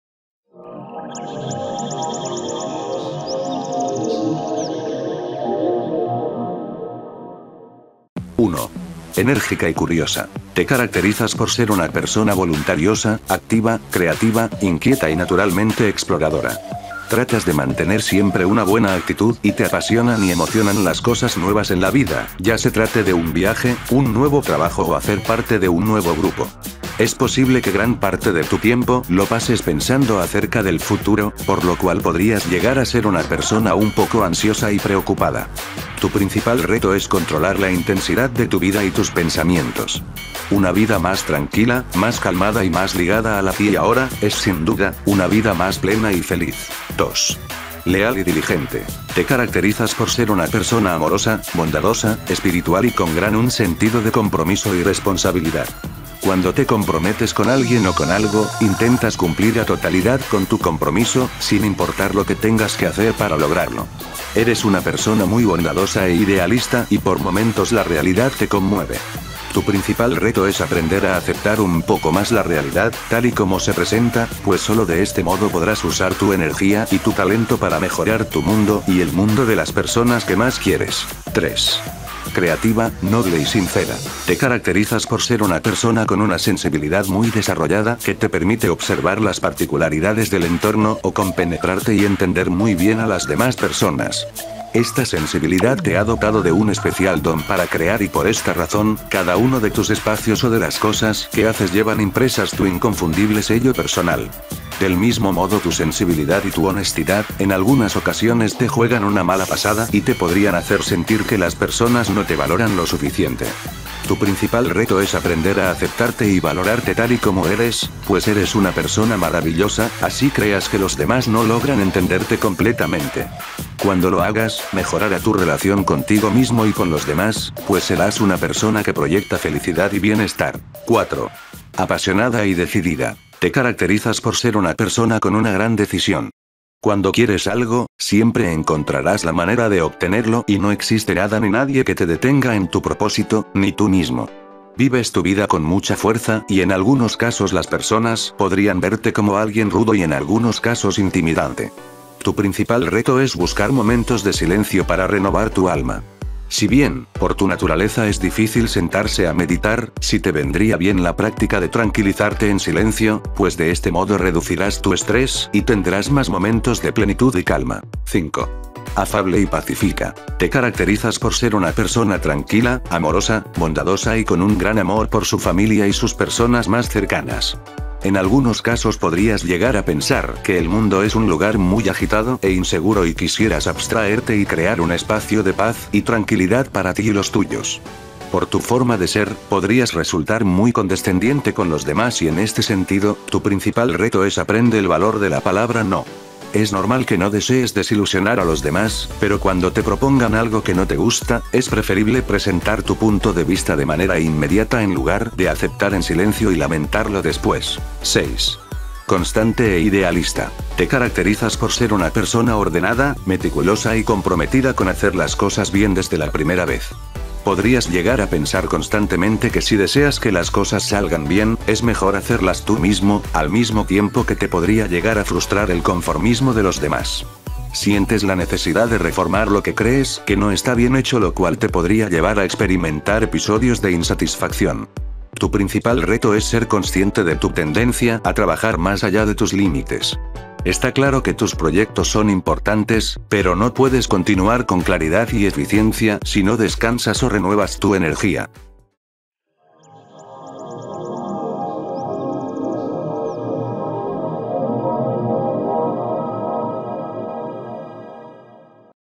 Elige una mano de Fátima y averigua el mensaje que tiene para ti
La mano de Fátima o hamsa es una figura protectora asociada a amparo, fortuna y fuerza. Del mismo modo, las características de tu hamsa favorita podrían representar aspectos de tu vida y tu personalidad.